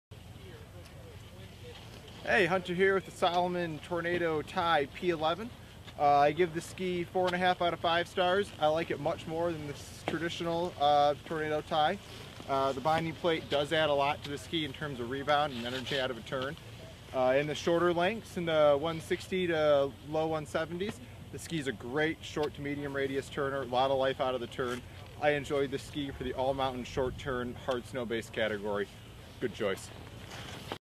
2011 Salomon X-Wing Tornado TI Powerline Skis Review from skis.com
Video review of 2011 Salomon X-Wing Tornado TI Powerline skis